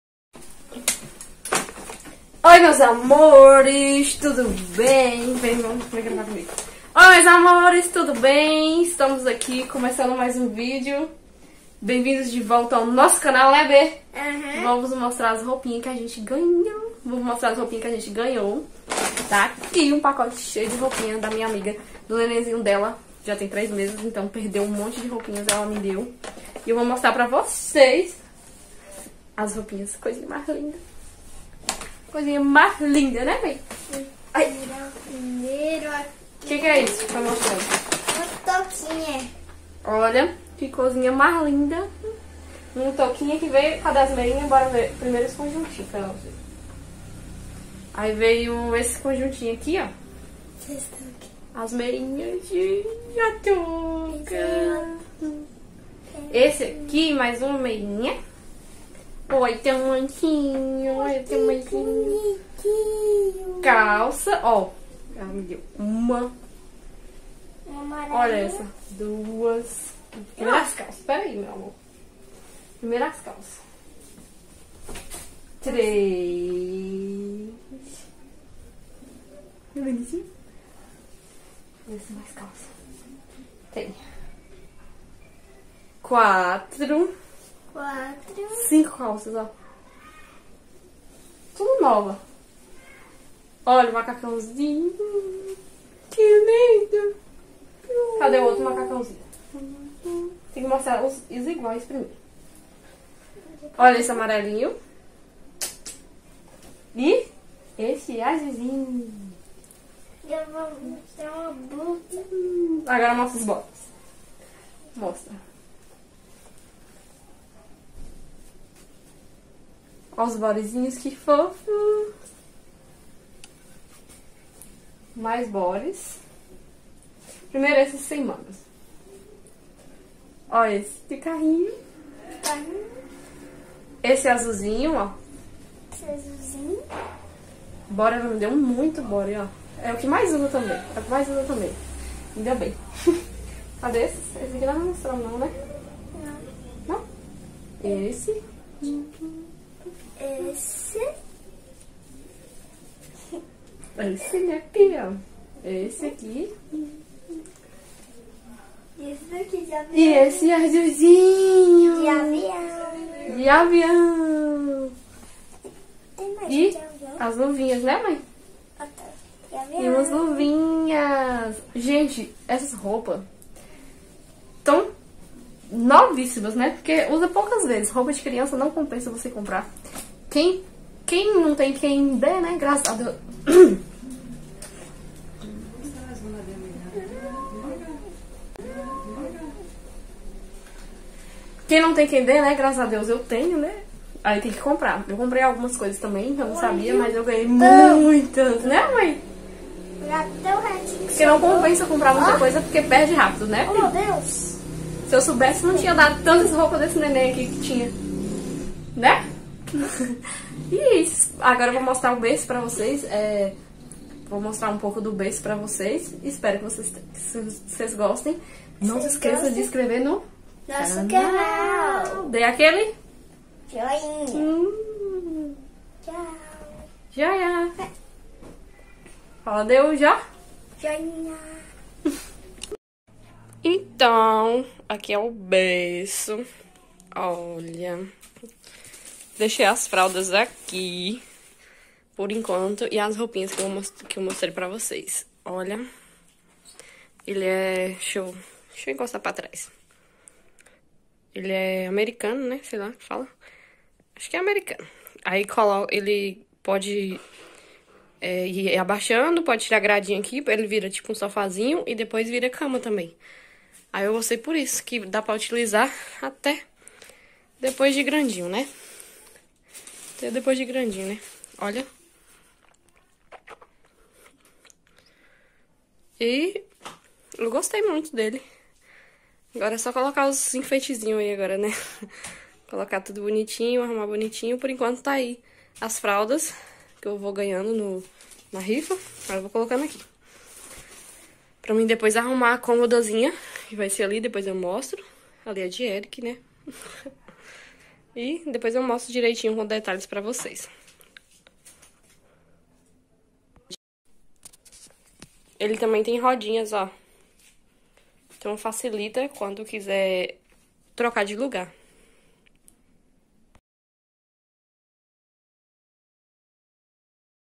GANHEI UM SACO DE ROUPAS SEMI NOVAS PARA O BEBE + TOUR PELO BERCINHO
GANHEI UM SACO DE ROUPAS USADAS PARA O BEBE E MOSTREI O BERCINHO